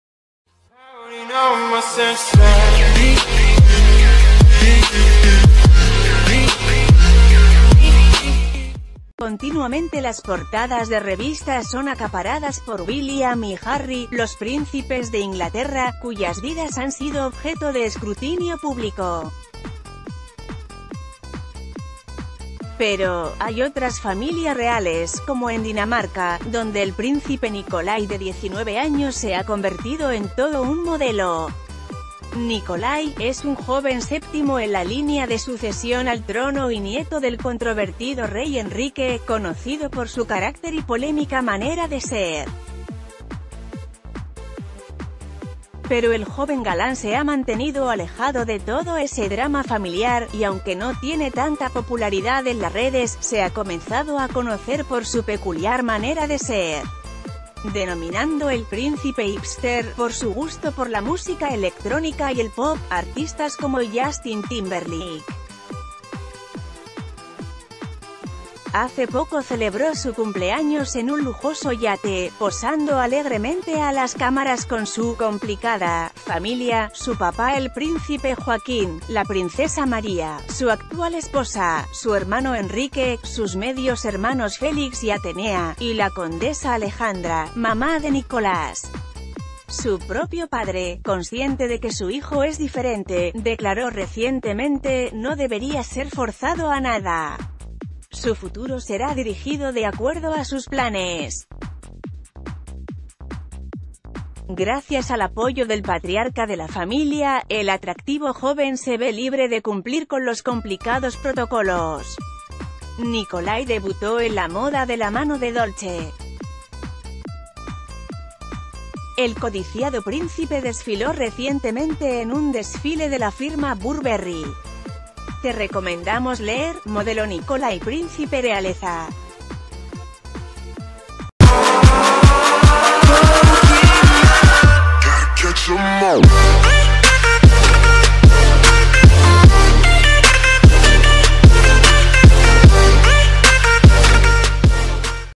Nikolai, el príncipe europeo que ahora es un modelo famoso
Continuamente las portadas de revistas son acaparadas por William y Harry, los Príncipes de Inglaterra, cuyas vidas han sido objeto de escrutinio público. Pero, hay otras familias reales, como en Dinamarca, donde el Príncipe Nikolai de 19 años se ha convertido en todo un modelo. Nikolai, es un joven...